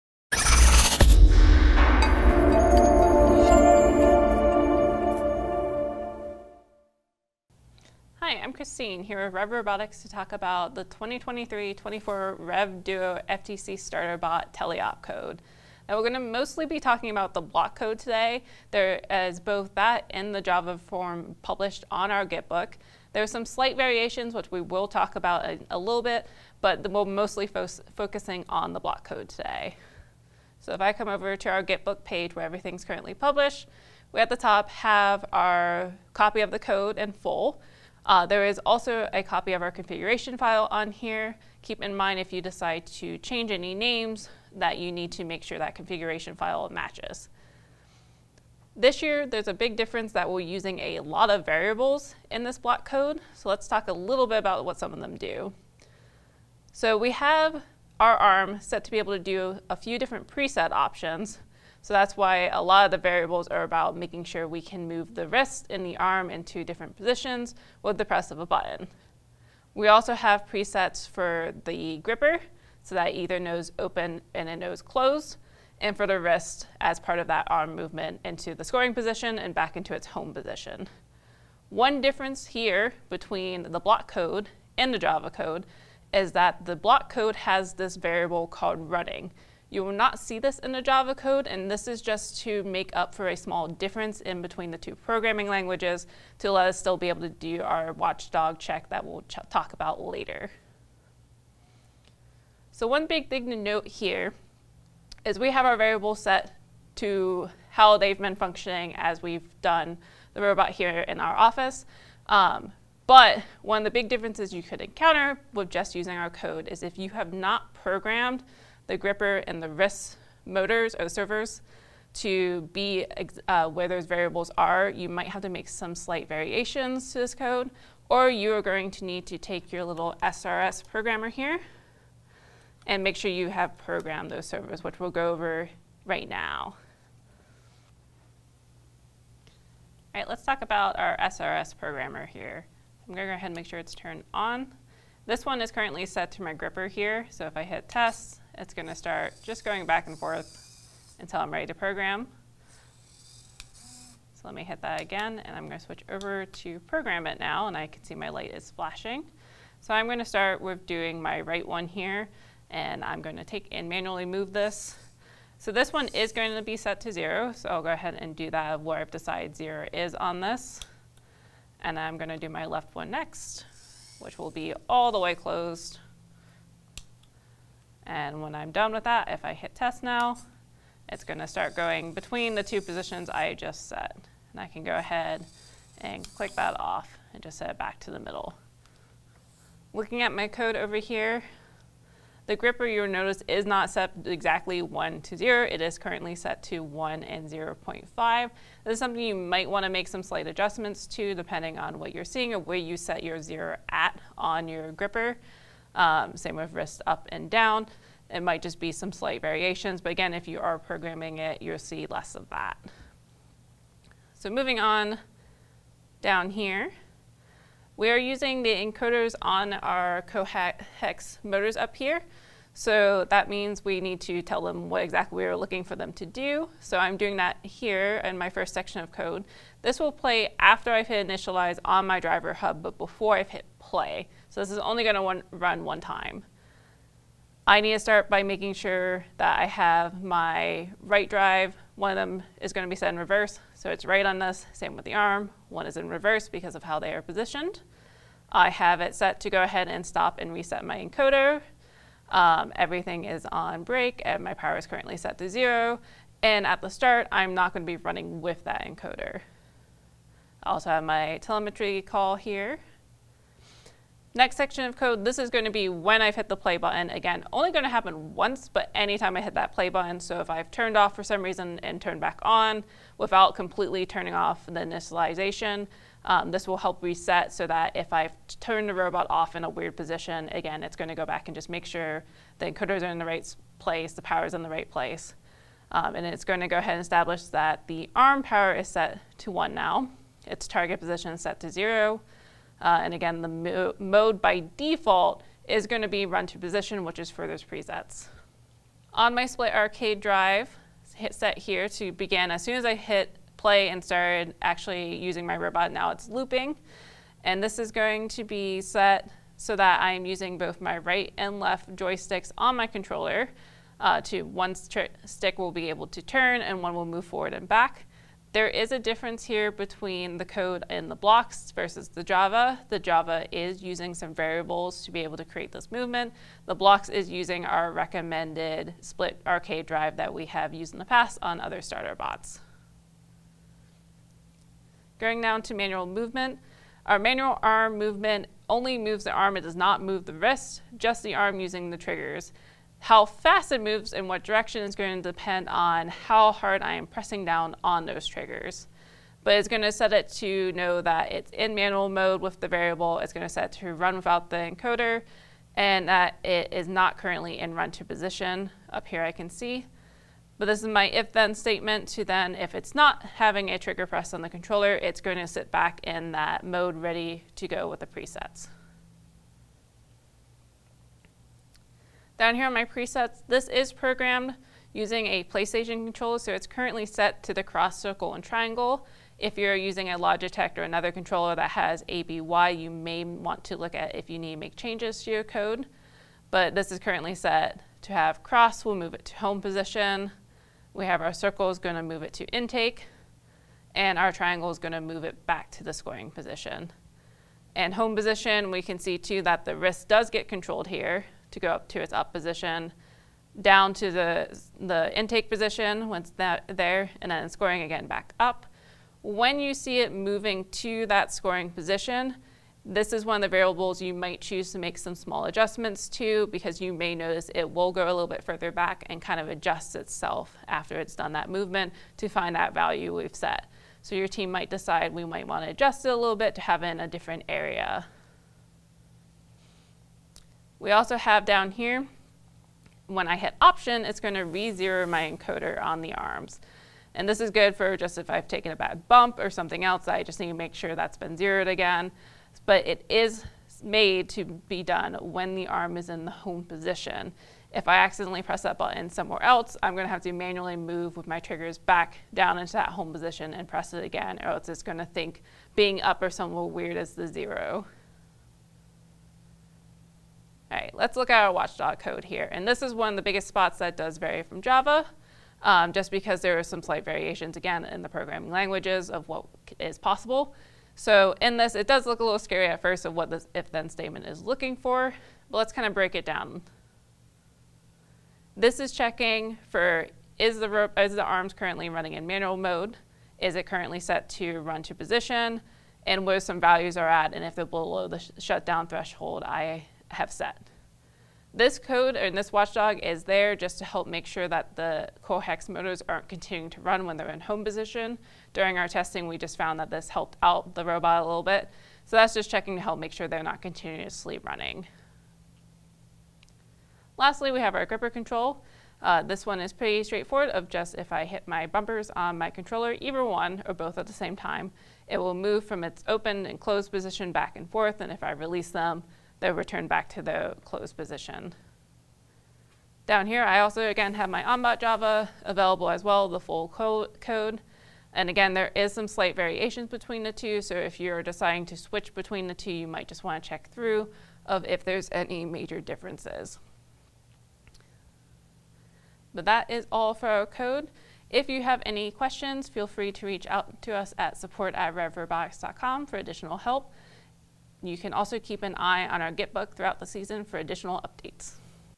Programming the 2023-24 REV DUO FTC Starter Bot
Learn how to get your 2023-24 REV DUO FTC Starter Bot configured and programmed for this year's game with Christine in this step-by-step instructional video!

For more information regarding configuring and programming your 2023-24 REV DUO FTC Starter Bot, please visit our FTC Kickoff Concepts technical documentation